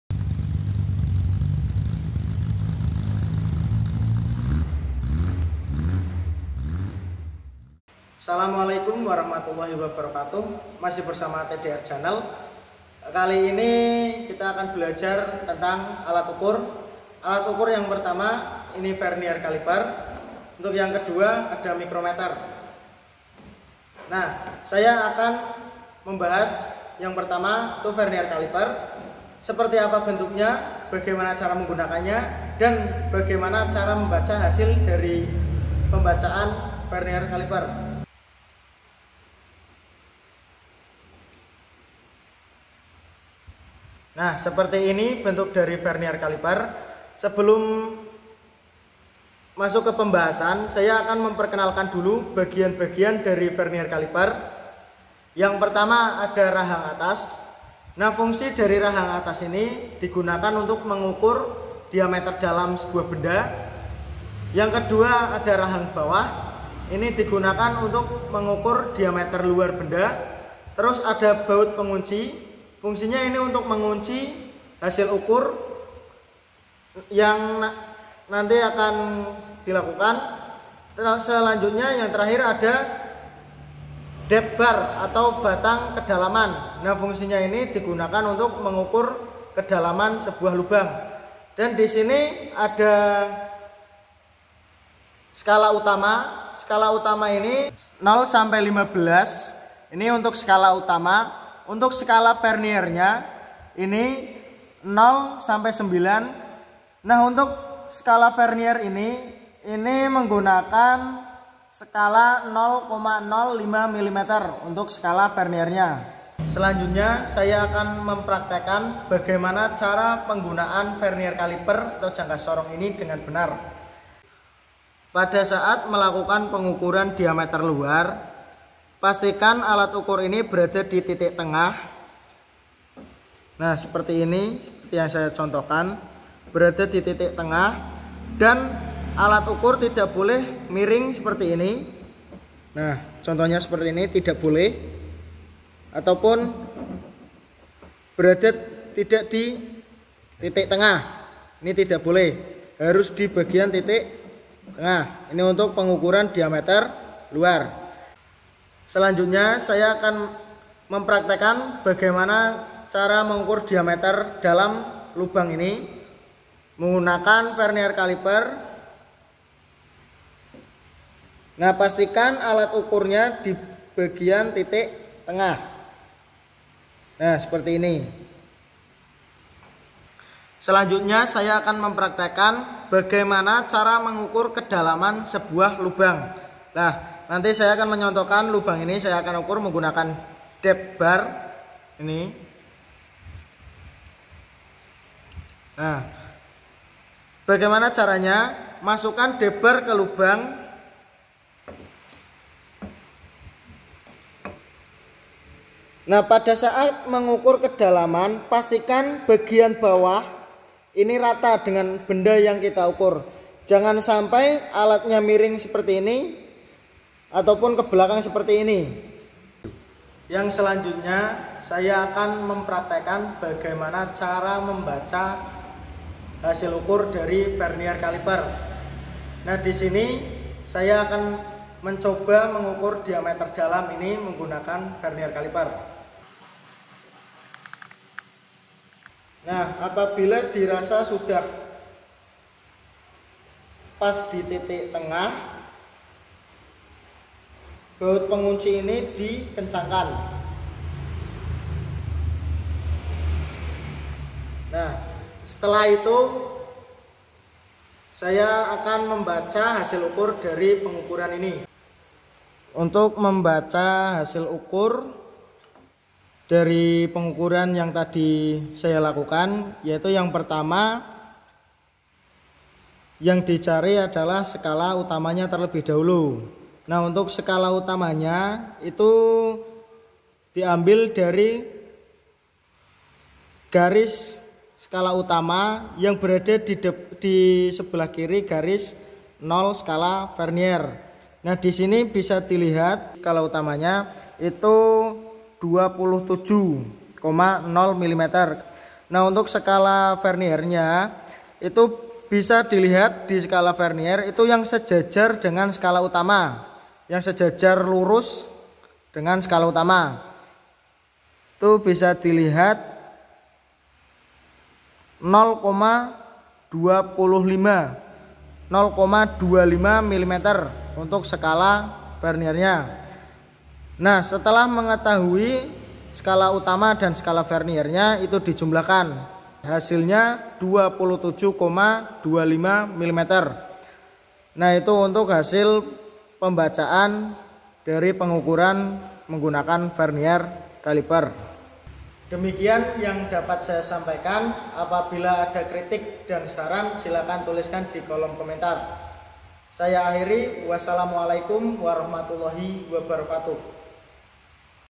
CARA MEMBACA HASIL UKUR JANGKA SORONG/VERNIER CALIPER & MICROMETER (PART 1)
Jumpa lagi dengan TDR Channel, kali ini kita akan berbagai ilmu tentang alat ukur jangka sorong dan micrometer, seperti apa bentuknya, apa fungsinya, bagaimana cara menggunakanya dan membaca hasil pengukuranya. mari kita simak video ini dengan seksama.
Terima kasih semoga bermanfaat, apabila aada kritik dan saran silahkan kita diskusikan di kolom komentar.